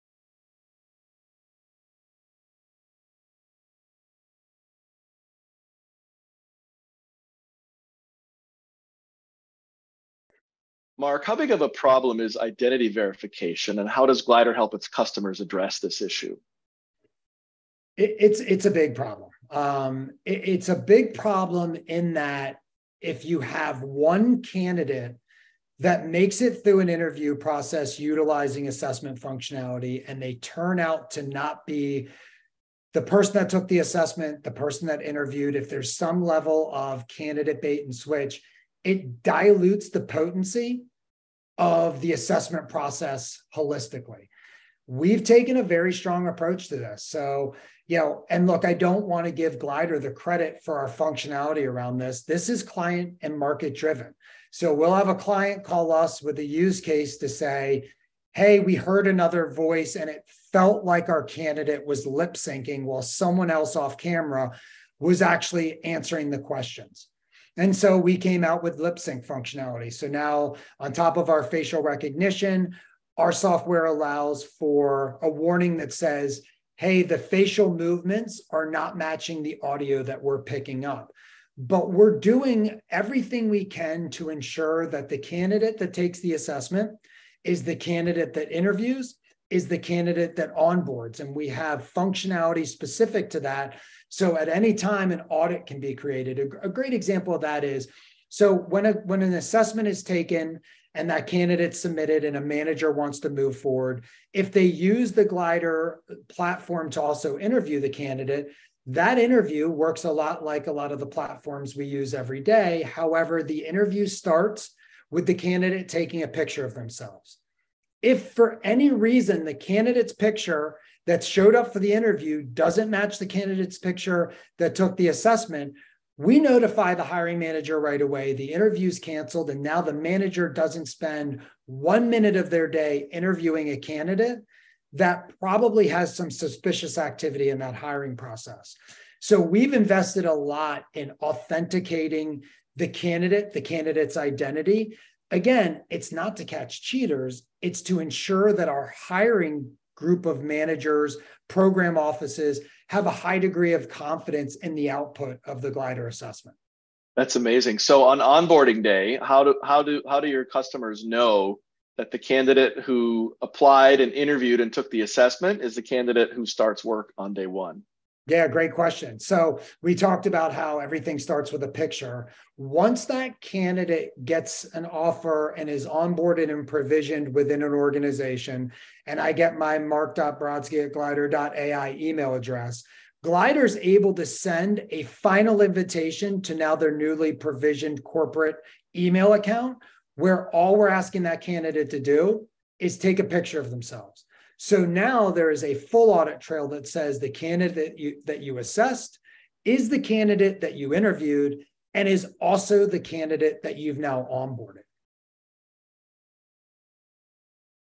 7/8 The importance of identity verification and how it’s done
Listen to part 7 of 8 in our new micro series on understanding talent trends, diving into AI, the evolution into skills-based hiring, and so much more as Jeff Mike and Mark Brodsky from Glider talk shop about the future of the extended workforce.